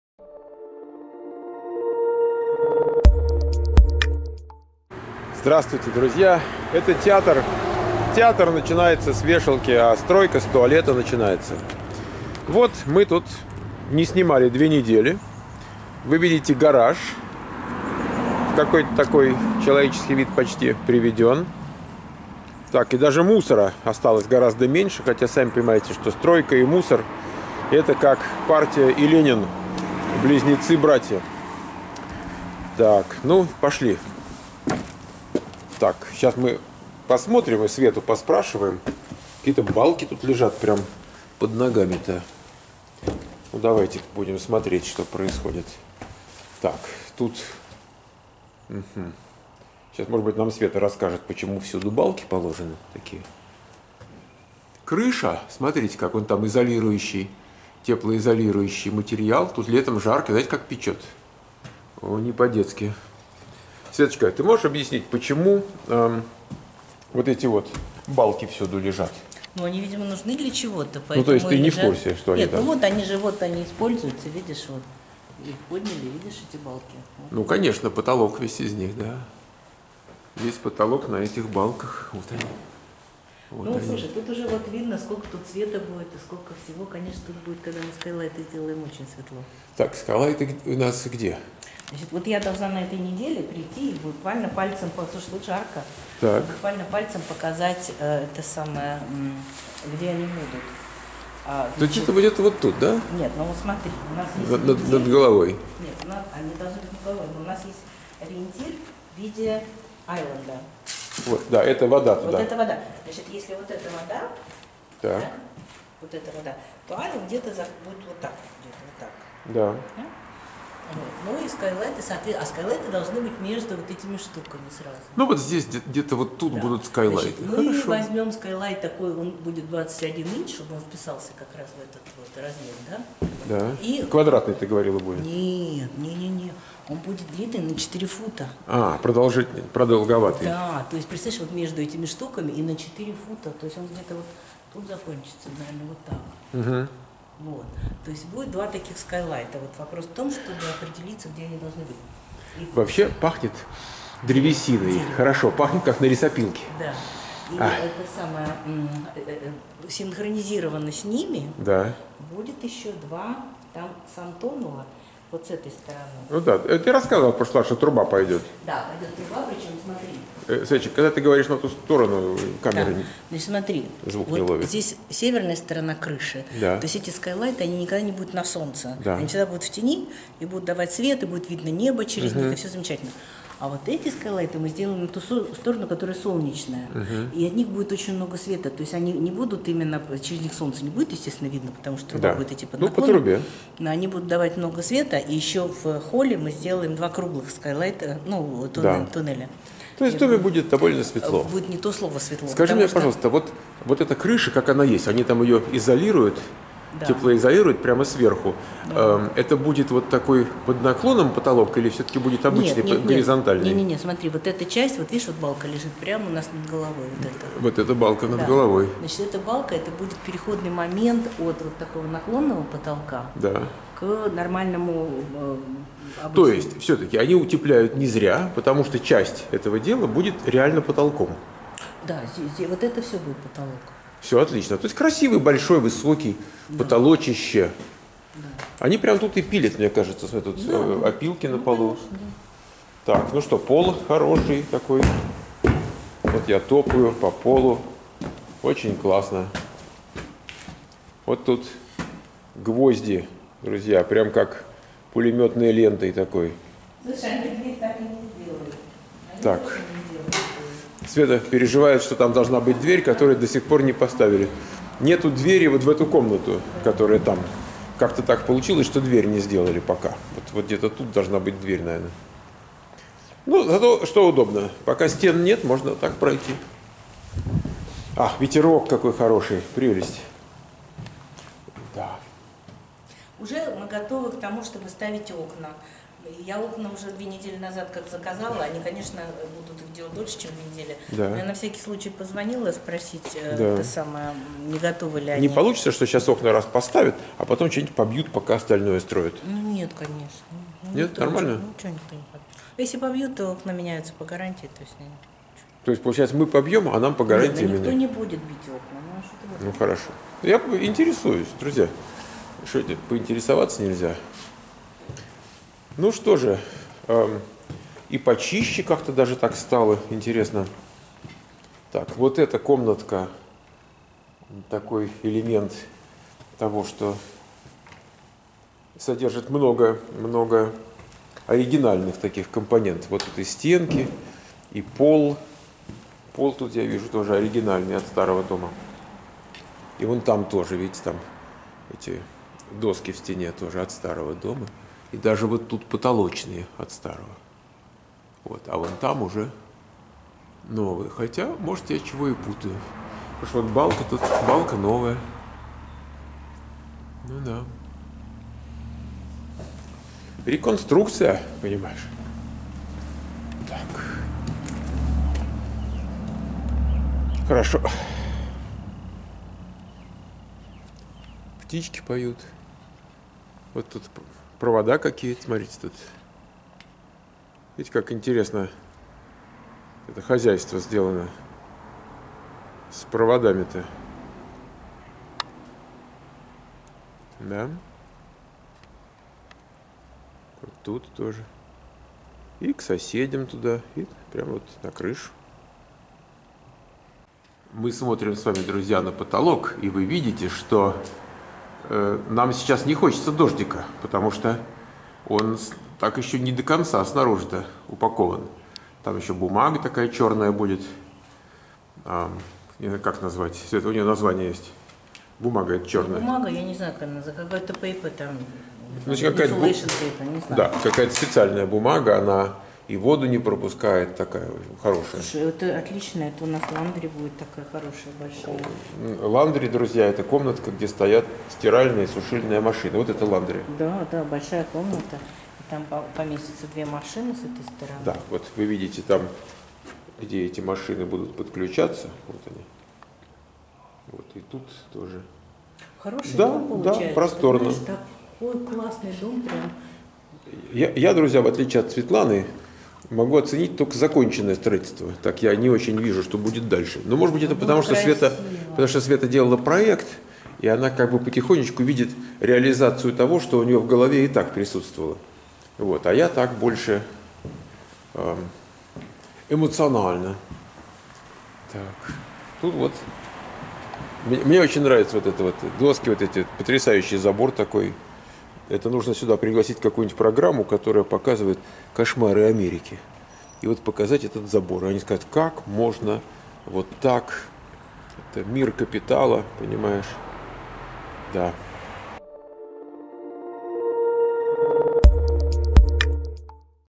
США 5084: Наша стройка - крыша есть и лето наступило. Пахнет лесопилкой. Смотрится очень красиво.
Наша стройка - крыша есть и лето наступило. Пахнет лесопилкой. Смотрится очень красиво.